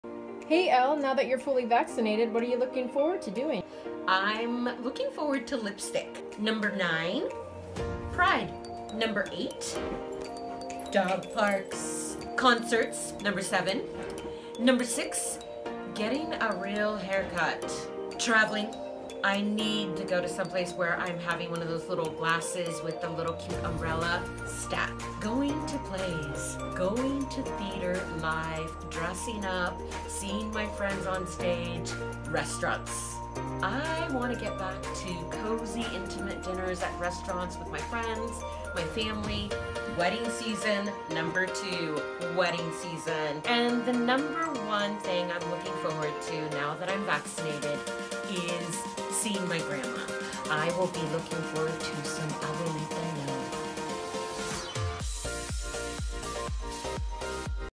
L’s top reason to look forward to
What are you looking for once you’re vaccinated? SFCreativeCorps Artist Leticia Duarte shared a list of the things they’re most excited about.

Artist: Leticia Duarte from San Francisco Bay Area Theatre Company

Artists inspire us when we need it most. This video was part of a series of public health messages shared by YBCA Community Health Ambassadors intended to inspire and keep us connected during the COVID-19 pandemic of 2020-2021.